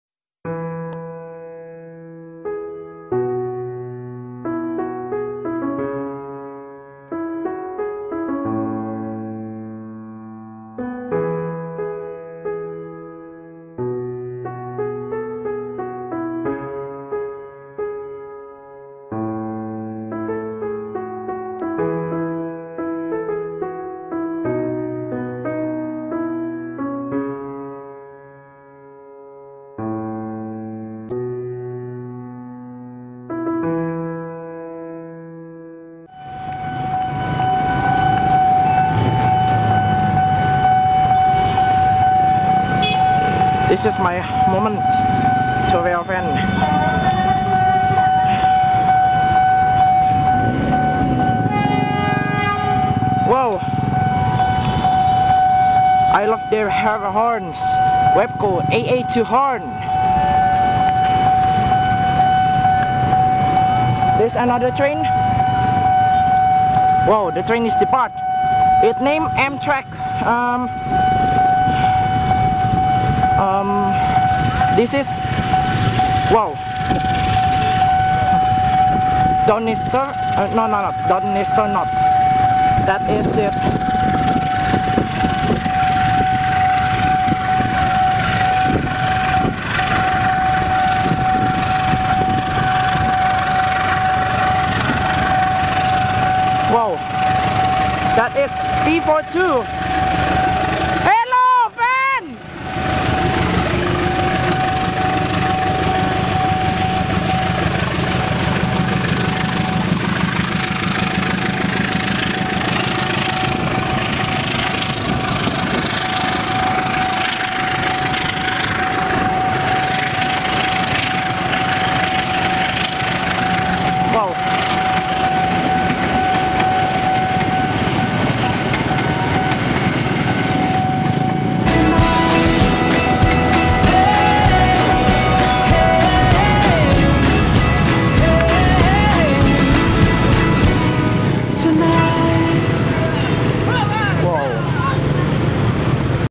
Capitol Limited with Amtrak #43 departs Union Station
Amtrak #43 leads Capitol Limited departs Union Station Washington, Washington DC. Amtrak 30 Capitol Limited with fouled K5LA that sound was like MU's airhorn Wabco AA-2